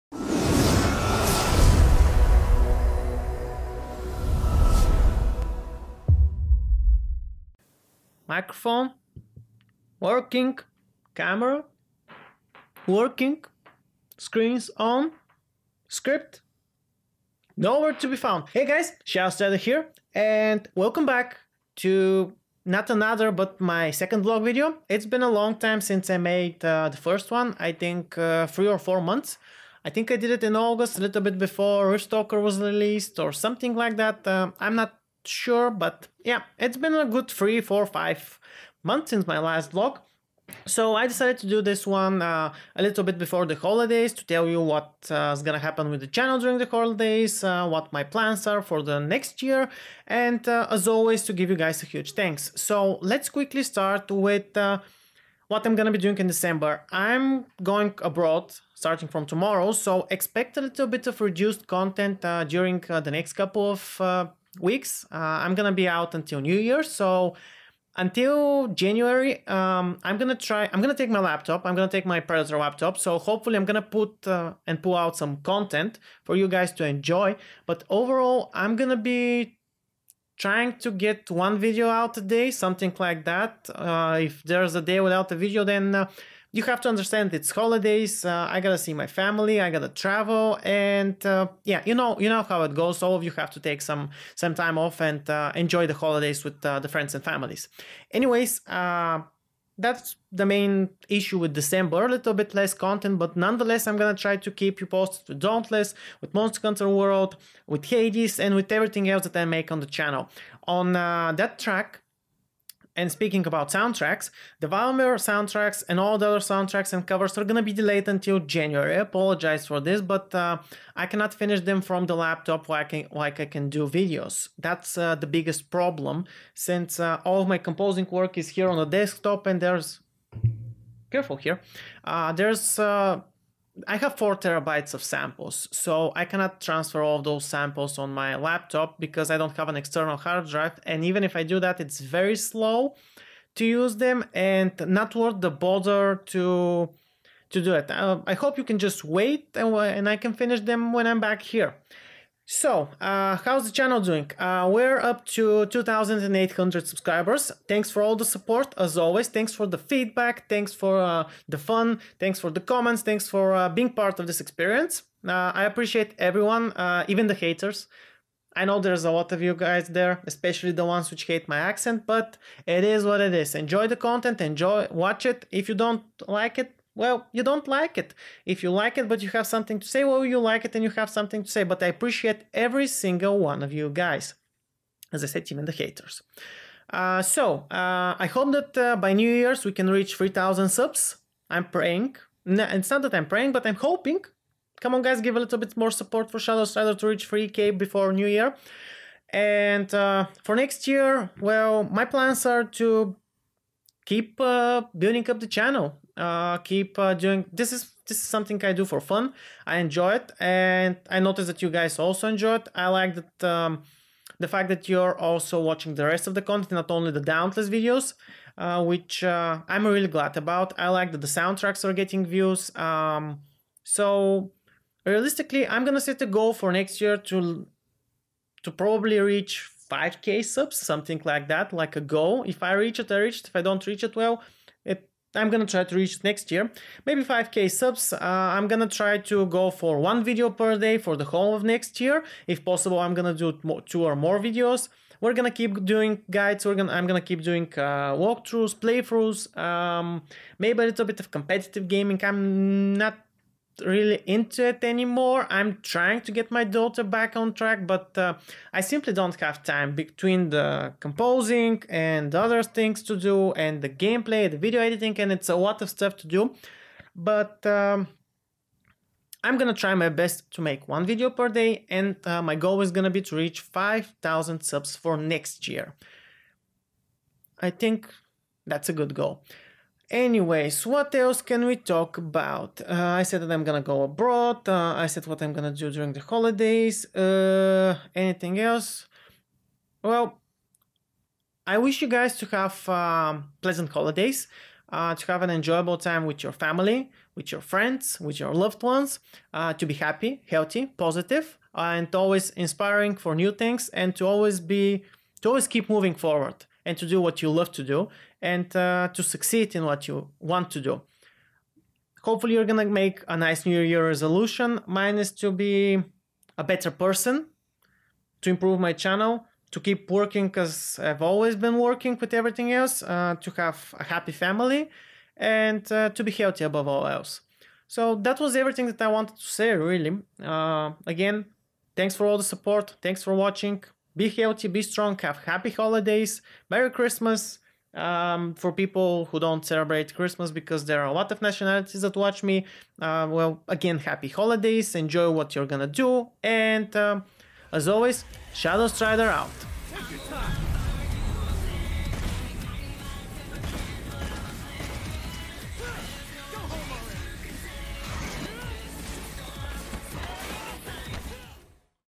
Vlog Video 2 - 2019 Goals, Holiday Content, Channel Update
A quick video vlog, to once again thank everyone who has supported me. I mention my goals for 2019 and wish you all happy holiday season!